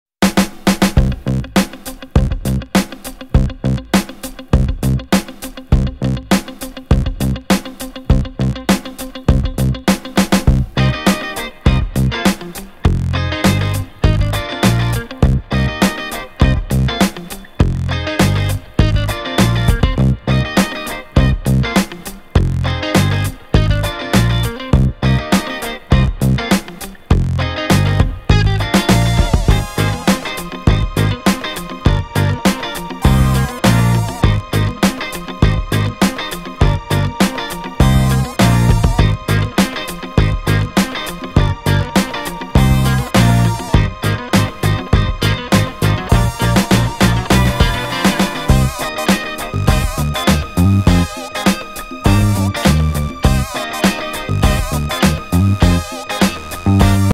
Where are batteries, fuses and relays in VW Phaeton (Volkswagen fusebox battery)
Battery, fuses and relays location in Volkswagen Phaeton car. There are two fuseboxes (under the dashboard and in the trunk) and two batteries (accessory and starter-only). Find out where are they so you can easily replace blown fuse and charge flat battery in your Volkswagen Phaeton car.